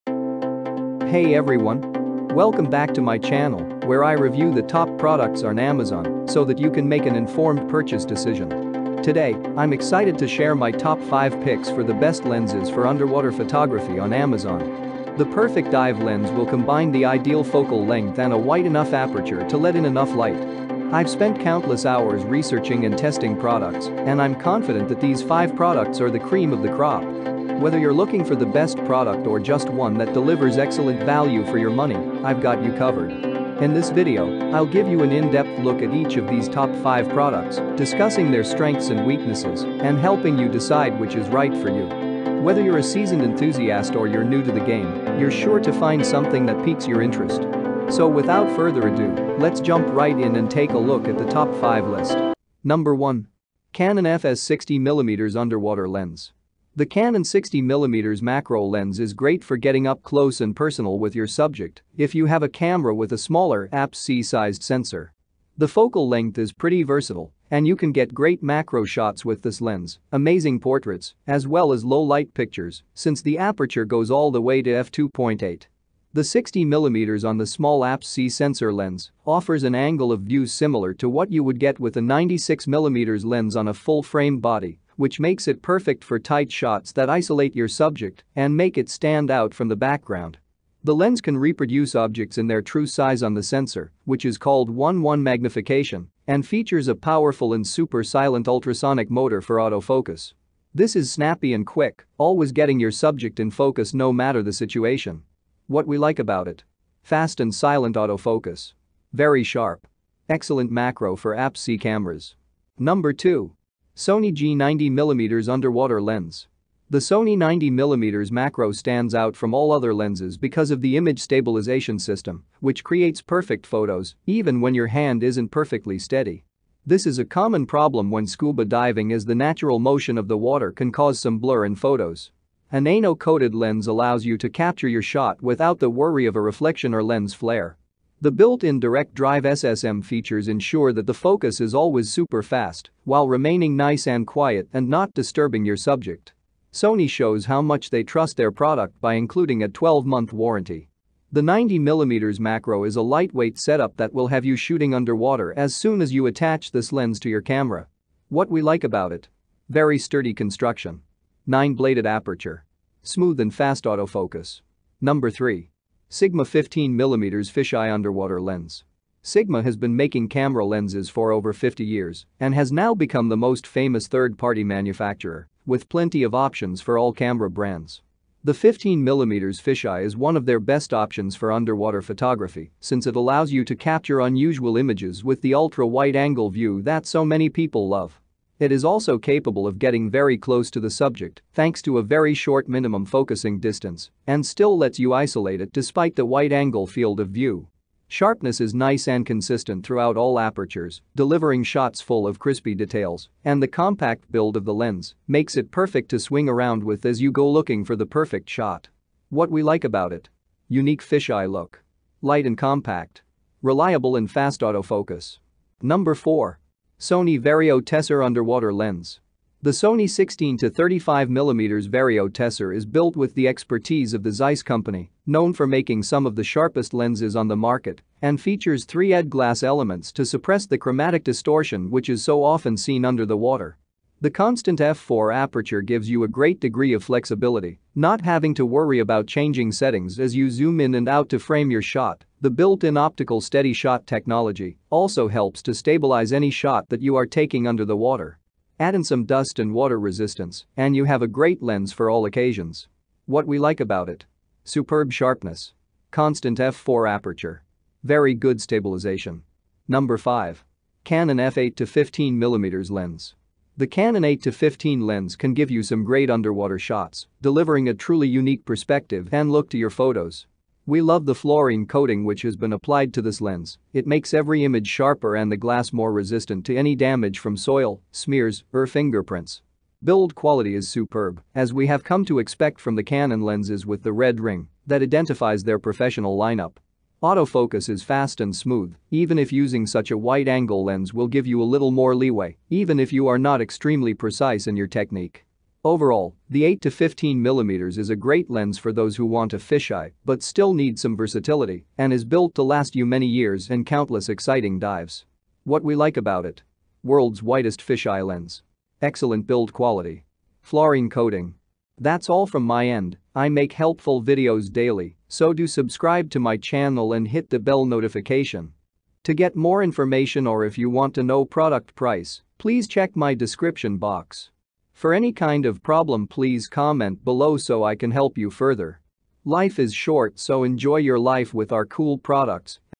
Capture the Deep: 5 Must-Have Underwater Photography Lenses!
👨🏻‍🔧Dive into the enchanting world of underwater photography with our latest video, "5 Best Lenses for Underwater Photography." Whether you're an avid diver or a photography enthusiast, choosing the right lens can make all the difference in capturing the vibrant and elusive beauty of the underwater realm.

In this comprehensive guide, we explore the top-notch lenses that have been carefully selected to enhance your underwater photography experience. Discover the extraordinary features and capabilities that set these lenses apart, enabling you to capture stunning images beneath the waves.

We'll take you on a journey through the world of underwater optics, highlighting the importance of factors like focal length, aperture, and image stabilization when choosing the perfect lens for your underwater adventures. Whether you're looking to capture intricate coral reefs, the graceful movements of marine life, or dramatic underwater landscapes, we've got you covered.

Join us as we showcase the impressive optical quality, durability, and ease of use that make these lenses ideal companions for your underwater photography endeavors. We'll also provide tips and tricks to help you maximize your results and unlock the full potential of your chosen lens.

📌Product Link📌
✅ My Picks ✅1.Canon EF-S 60mm Underwater Lens
✅ Best Value ✅2.Sony G 90mm Underwater Lens
✅ For Beginners ✅3.Sigma 15mm Fisheye Underwater Lens
✅ Premium ✅4.Sony Vario-Tessar Underwater Lens
✅ Trending ✅5.Canon EF 8-15mm Lens